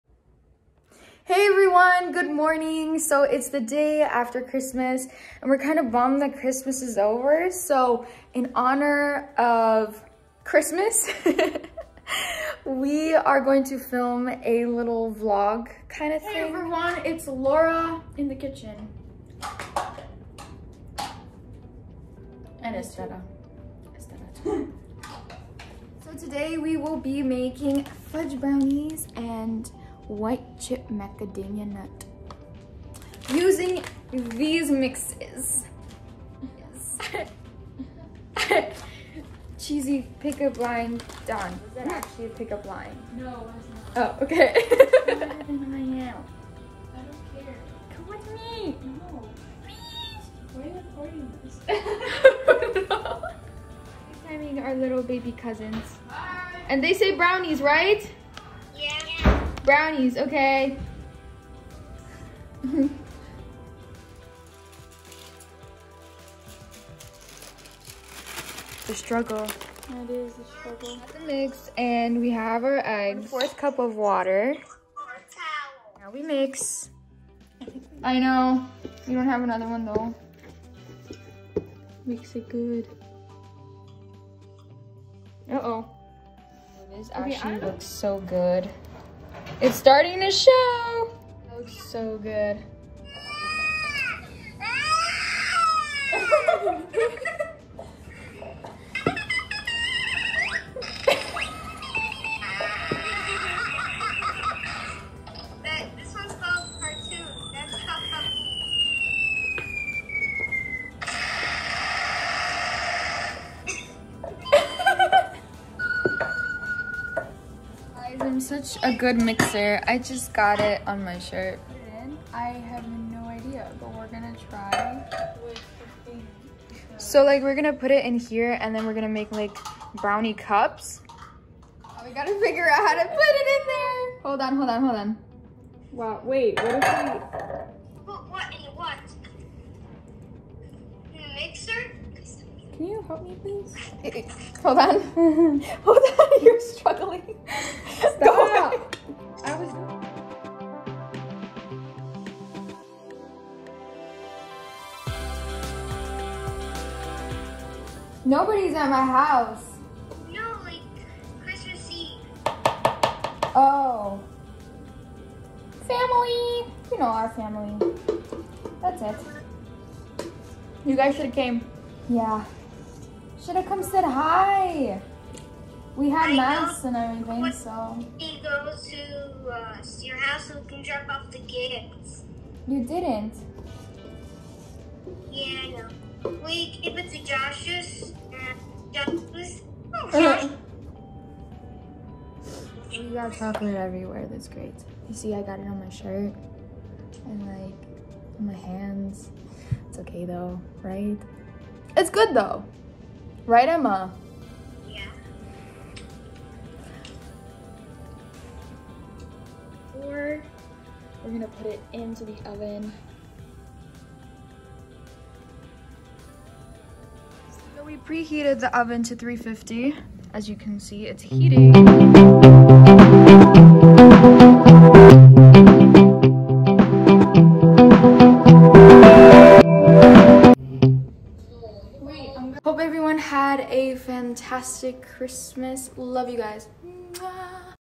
Laura Bretan - Making Christmas Brownies | Vlog
Me and my sister in the kitchen. Watch out! 😍